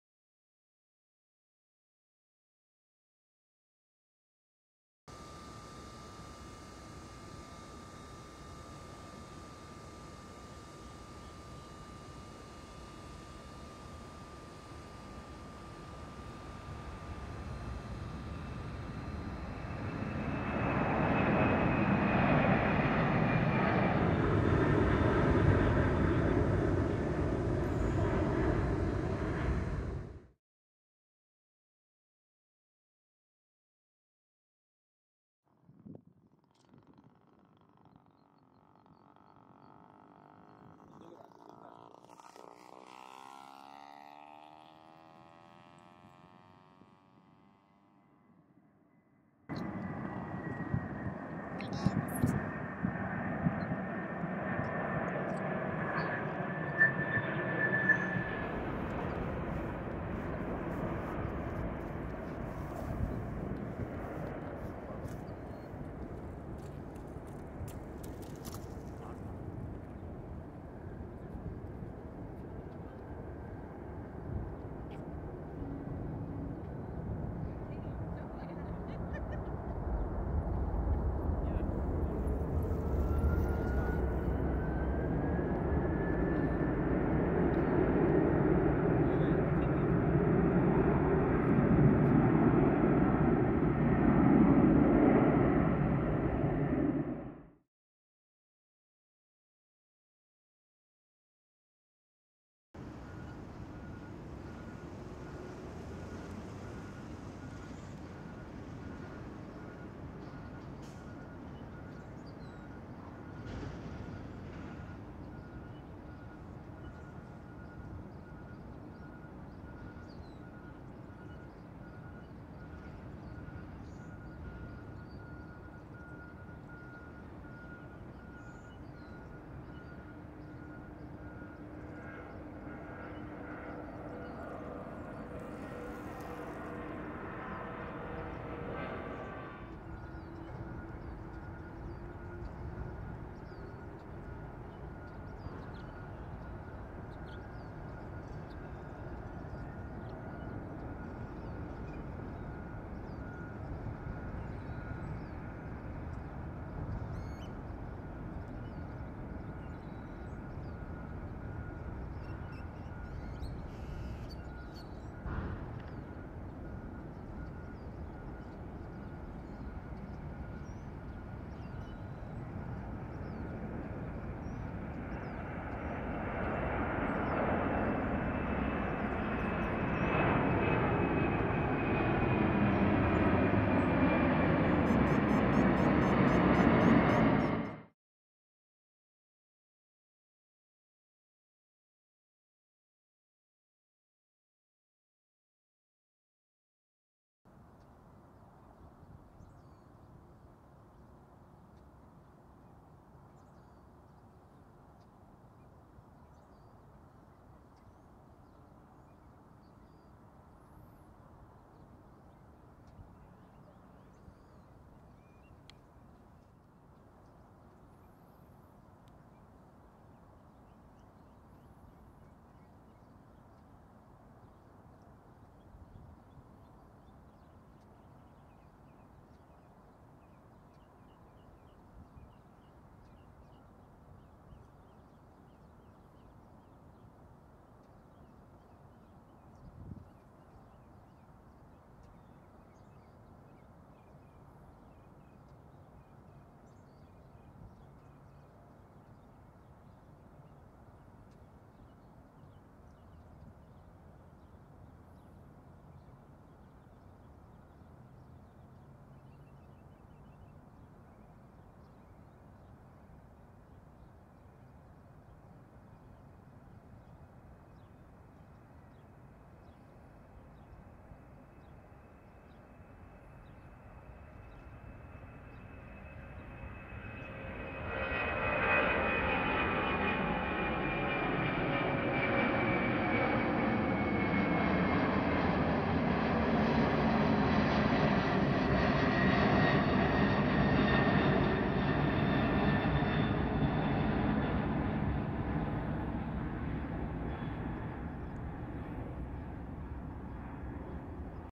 (4k) MD-11's and DC-10 @ PHX.
1 Landing and 2 Takeoff's of MD-11's.
1 Takeoff of a 47 Year old DC-10.